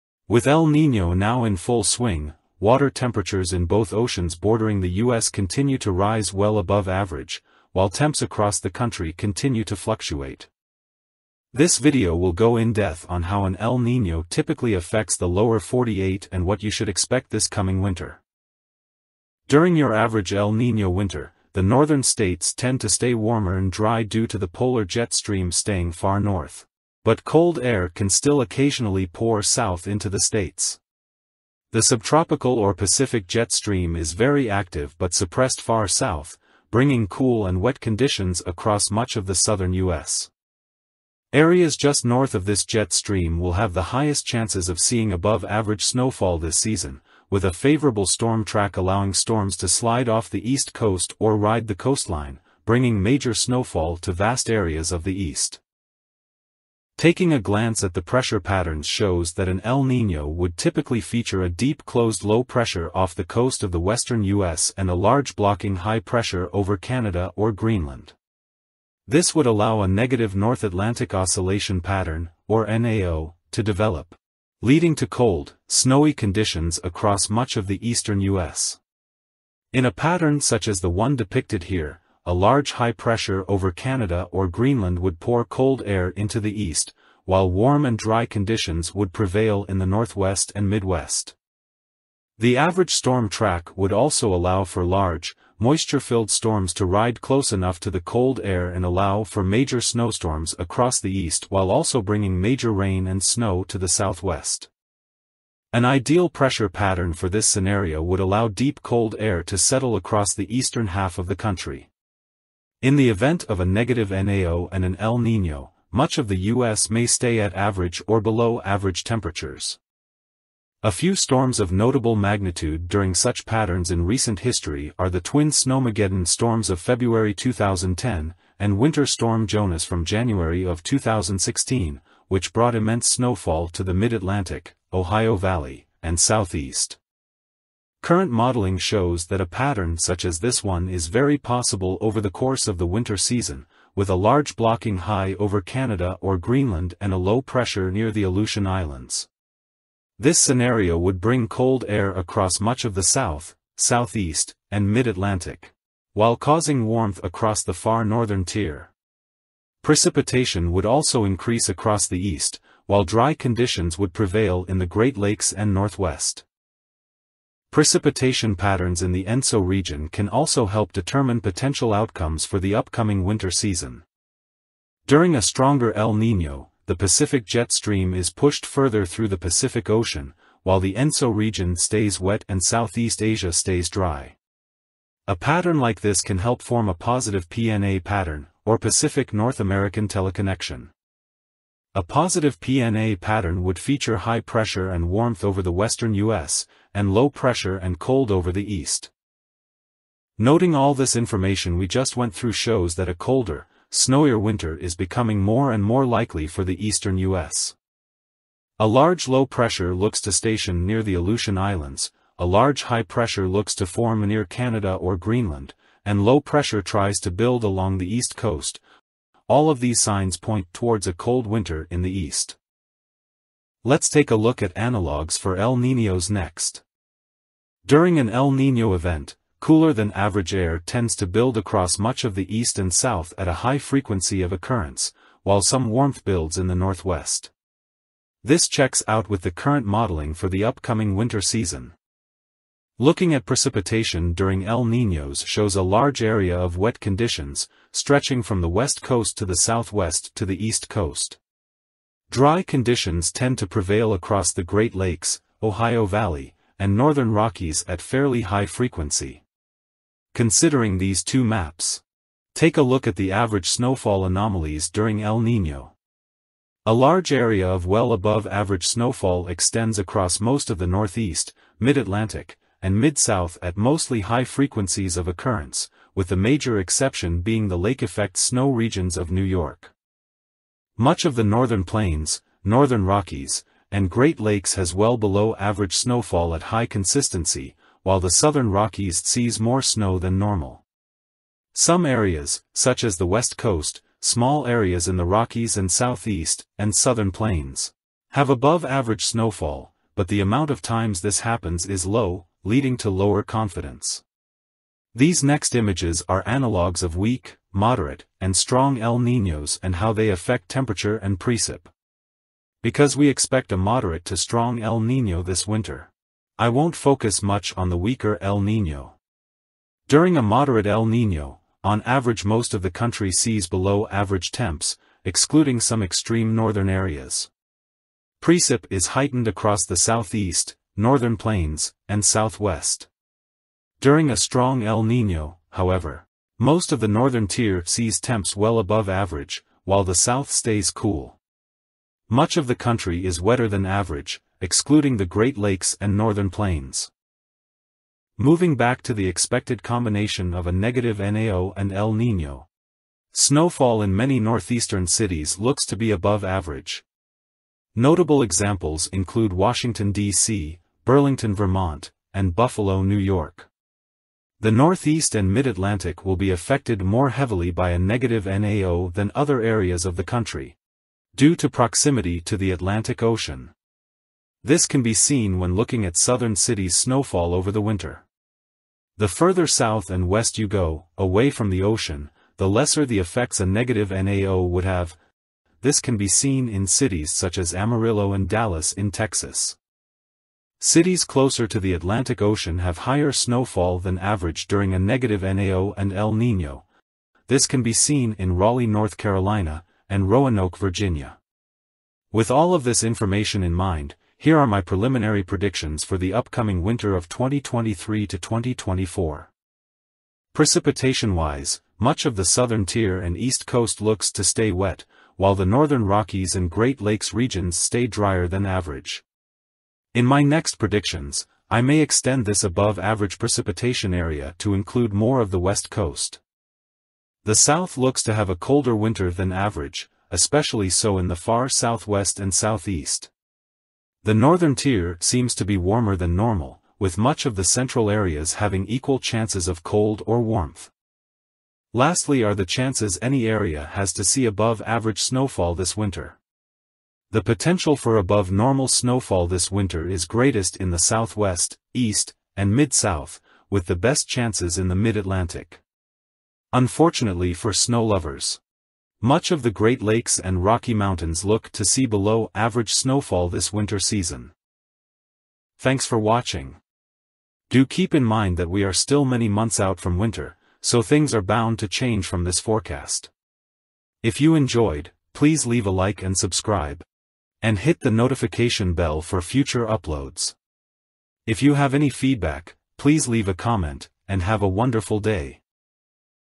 Winter 2023-2024 Forecast | What El Niño Means for the Upcoming Winter
In this video we analyze the potential effects of El Niño and other teleconnections on the upcoming Winter of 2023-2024, as well as what this winter season may have in store for the US.